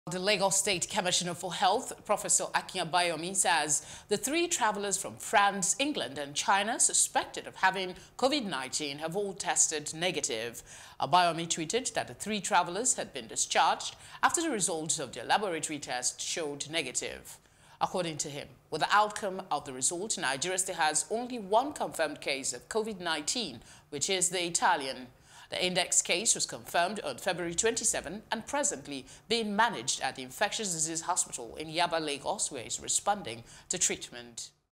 Convid19 Update: 3 Suspected Cases In Lagos Tested Negative - Prof Akin
Meanwhile, The Lagos State Commissioner for Health, Prof. Akin Abayomi, says the three travelers from France, England and China suspected of having COVID-19 have all tested negative.